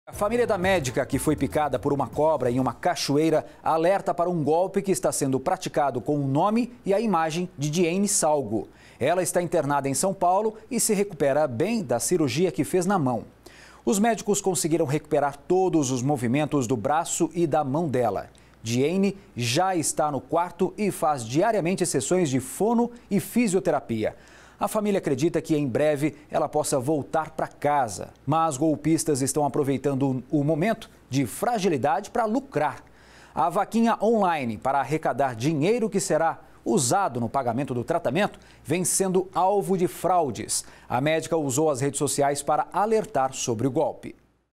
Médica picada por cobra se recupera de cirurgia
A médica Dieynne Saugo que foi picada por uma cobra em Mato Grosso passou por uma nova cirurgia no hospital Albert Einstein, na zona sul de São Paulo.